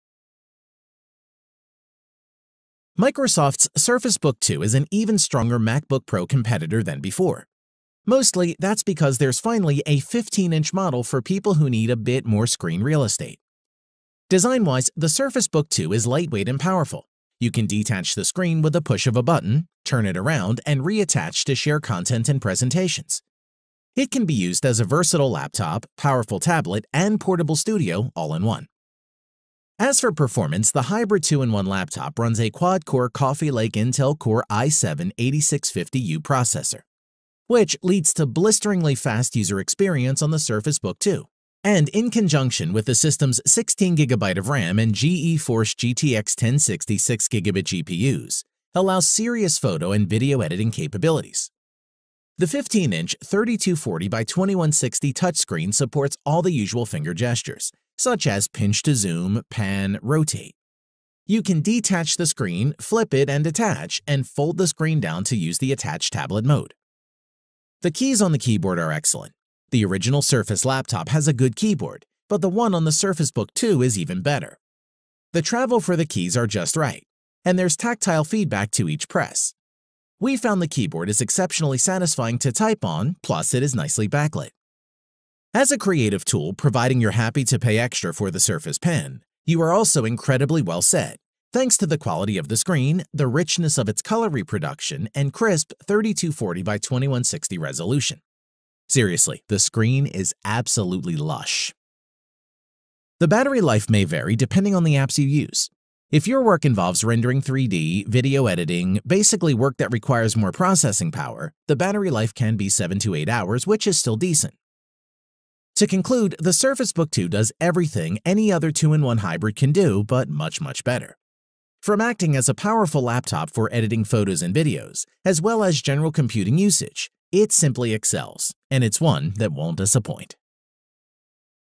Portable studio all-in-one Microsoft Surface Book 2 Review and Specification
Surface Book 2 is the most powerful Surface laptop ever; built with power and versatility to be a laptop, tablet, and portable studio all-in-one.

Buy
Microsoft Surface Book 2